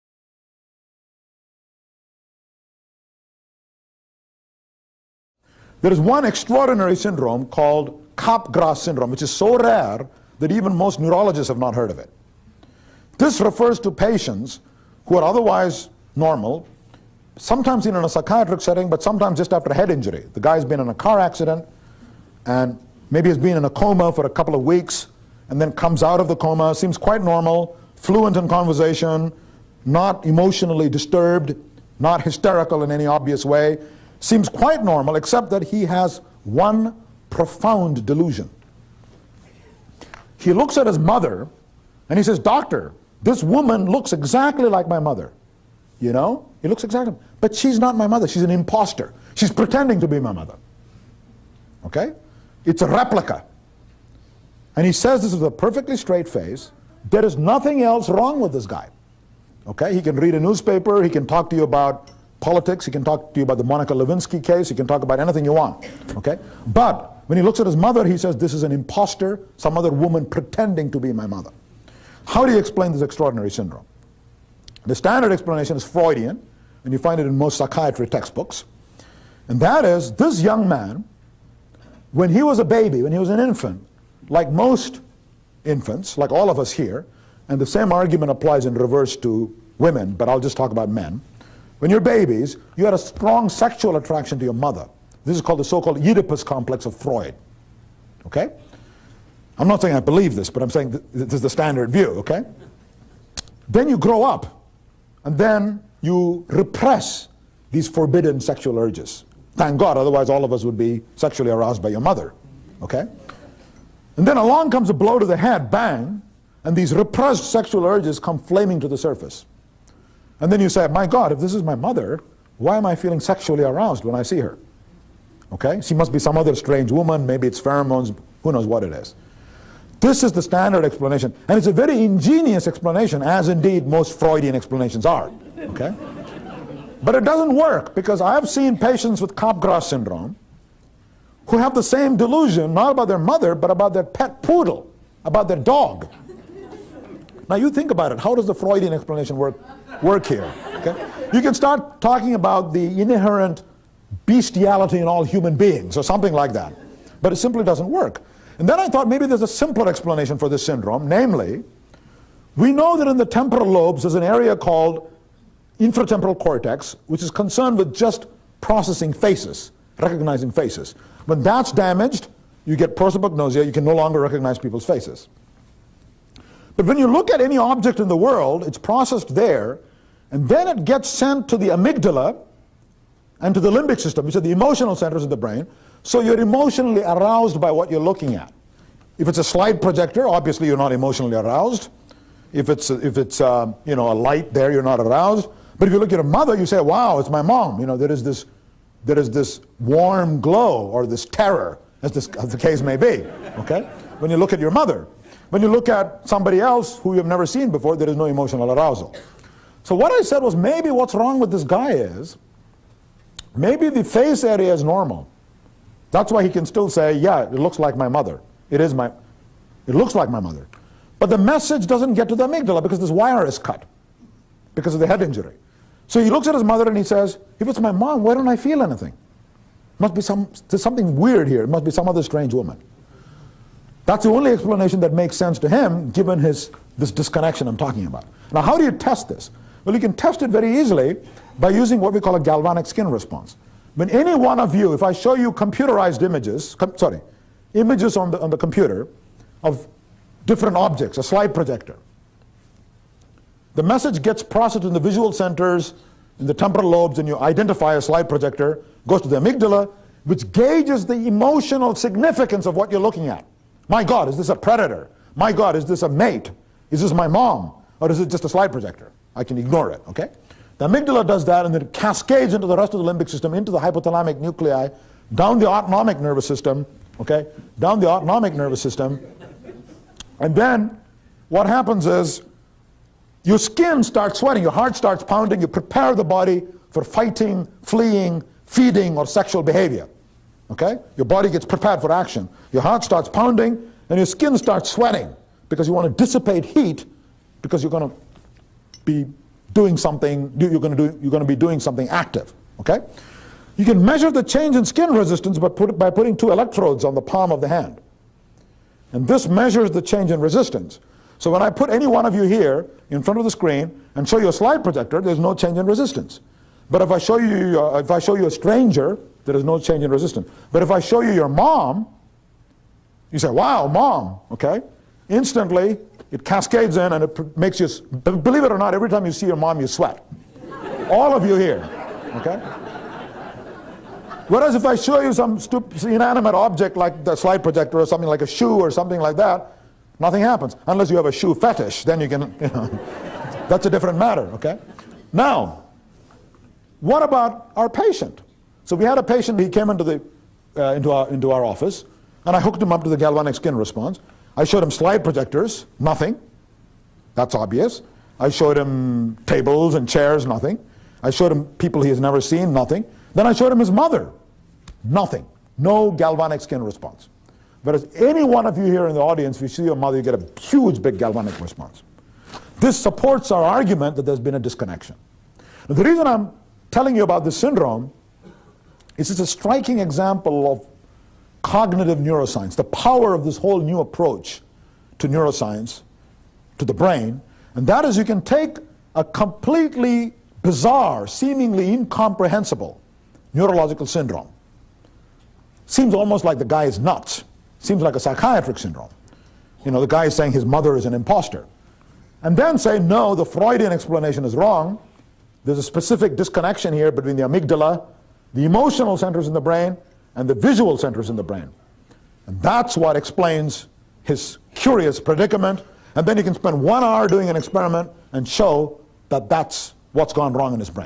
"My Mother is an Impostor" - V. S. Ramachandran explains bizarre delusion
V.S. Ramachandran is Director of the Center for Brain and Cognition at the University of California, San Diego. He has gained wide recognition for uncovering the neurological basis for a host of bizarre psychological conditions.

His books include:
The Tell-Tale Brain: A Neuroscientist's Quest for What Makes Us Human
A Brief Tour of Human Consciousness: From Impostor Poodles to Purple Numbers
Phantoms in the Brain: Probing the Mysteries of the Human Mind

This video is excerpted from his presentation "Neurology and the Passion for Art".